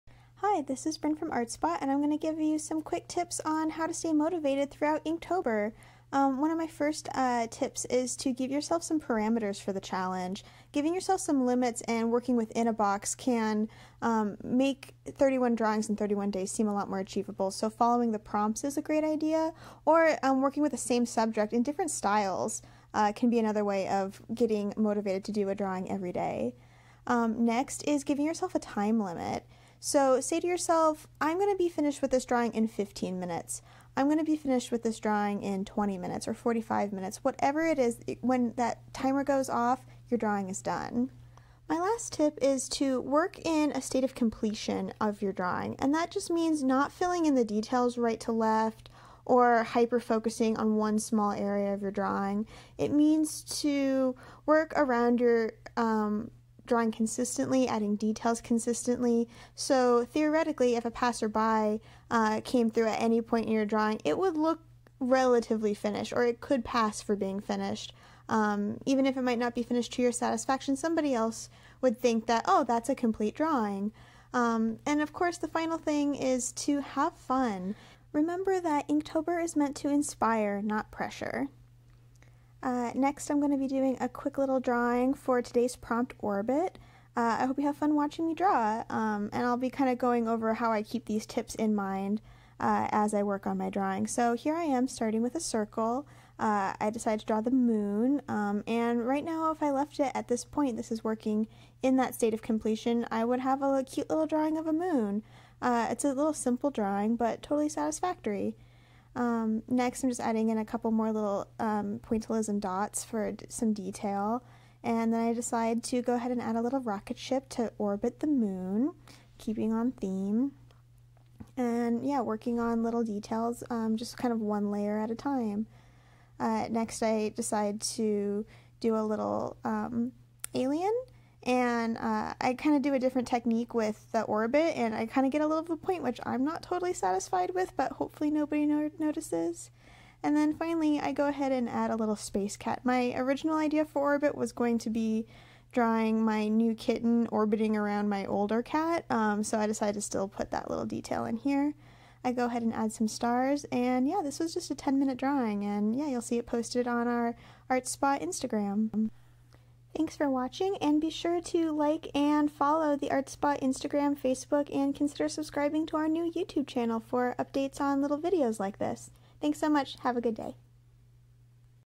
Quick Tips for Sticking With the Inktober Challenge!
Quick little video offering some tips on how to stick with the Inktober Challenge! Who knew drawing 31 drawings in 31 days was so hard!?

Host & Editing by Bryn Alberto